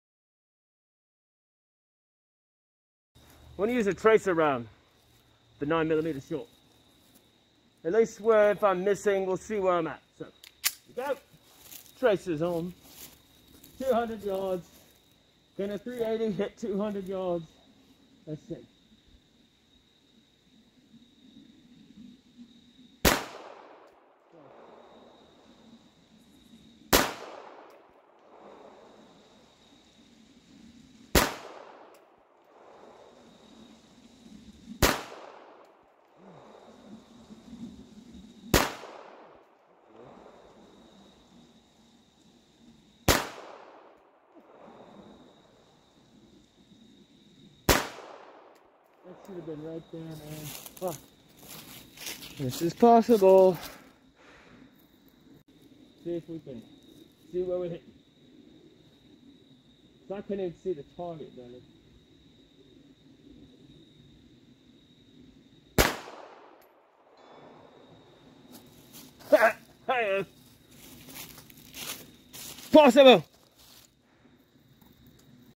Can a 380 Tracer Round Hit Steel At 200 Yards ?
Hitting a steel target at 200 yards with a 380 auto, shooting with a Walther pk380 using tracer rounds